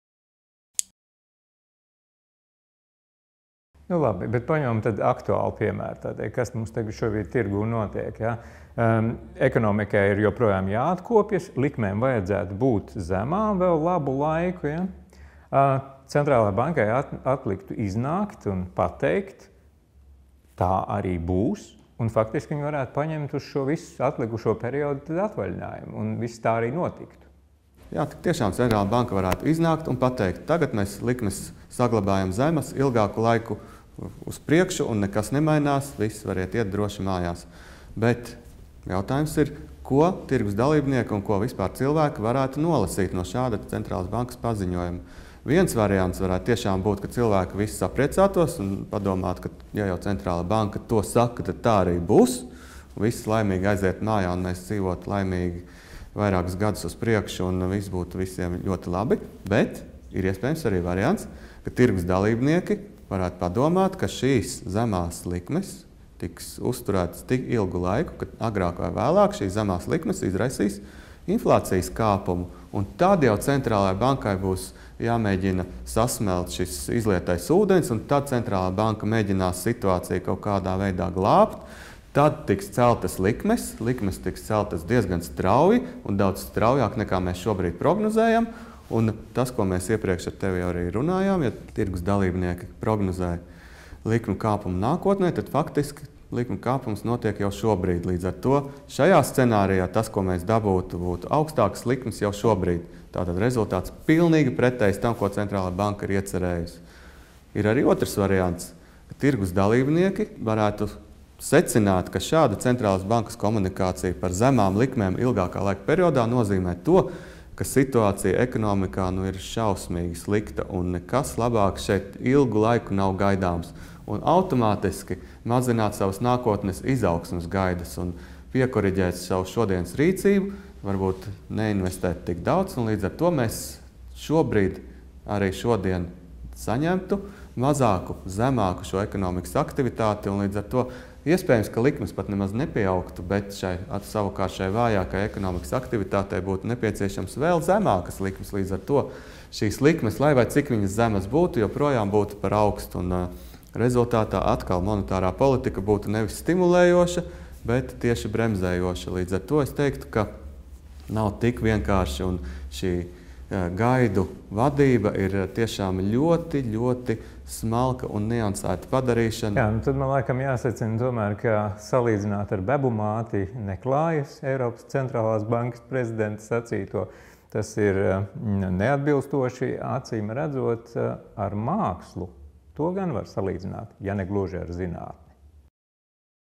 5. Iespējamās reakcijas uz gaidām
Atkarībā no tirgus dalībnieku ticības centrālo banku teiktajam izskanējušās ziņas var tikt uztvertas pēc dažādiem scenārijiem.